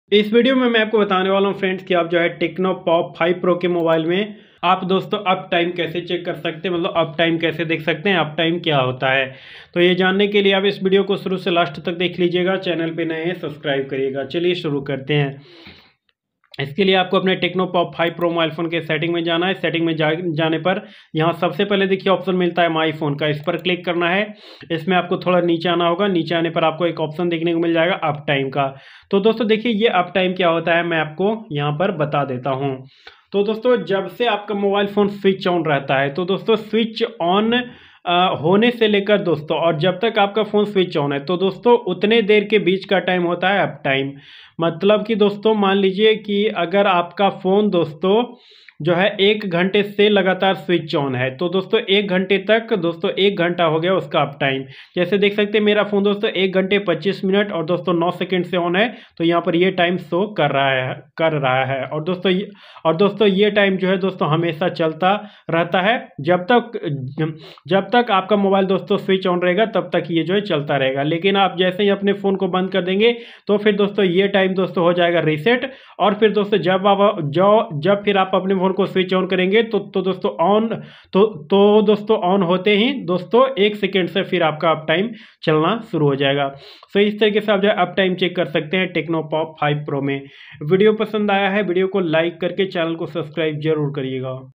Tecno Pop 5 pro me up time kaise check kare ya dekhe ।how to check up time in Tecno Pop 5 pro
is video me bataya gaya hai friends ki Tecno Pop 5 pro me up time kaise check kiya jata hai